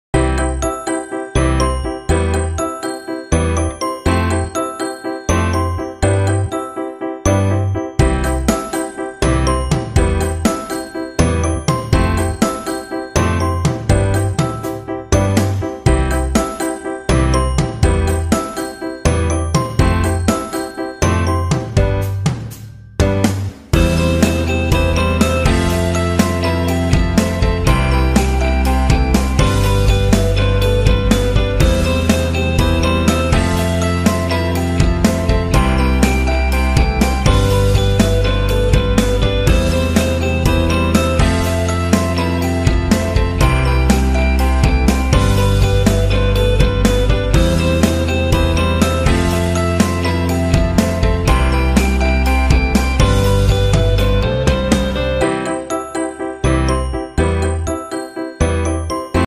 QUICK DIY LOVE CARD
Welcome to @Handmadecardsideas In this video I am going to show you guys how to make quick love card.